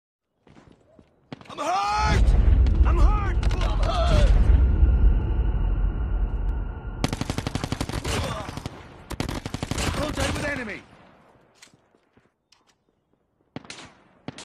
best gaming chair 😥
Hey ninjas it Ur big ninja here , this video is a hacker while we were playing championship so my friend recorded only this much so yea njoy subscribe and leave a like thxs see y'all in the next one ✌️